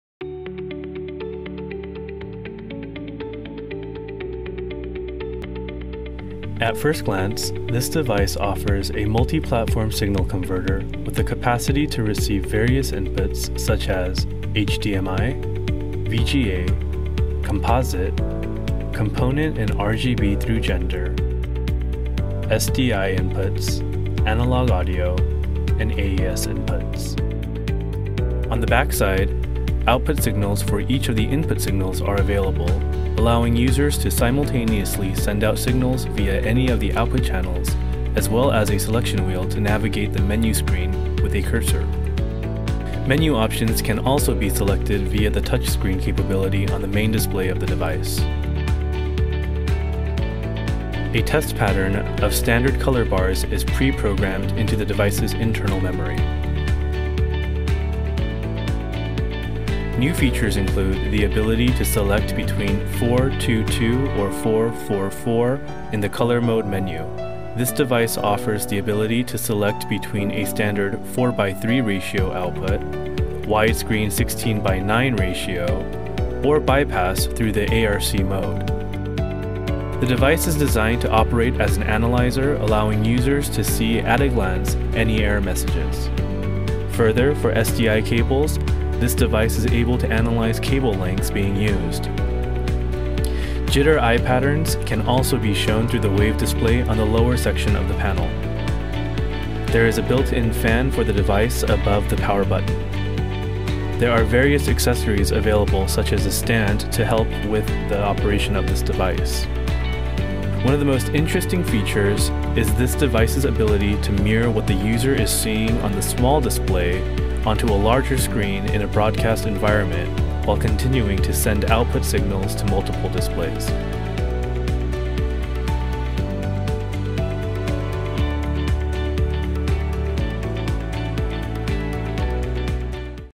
DIGITAL FORECAST BRIDGE X TS
Digital Forecast Bridge X-TS supports a variety of inputs and outputs and it can be used in the installation environment and in maintenance service of AV signal.
It is signal analyzer, test pattern, Up/Down/Cross/Scan converter w/ 5" touch screen (Mirroring function thru HDMI and Analog audio Mux/Demux)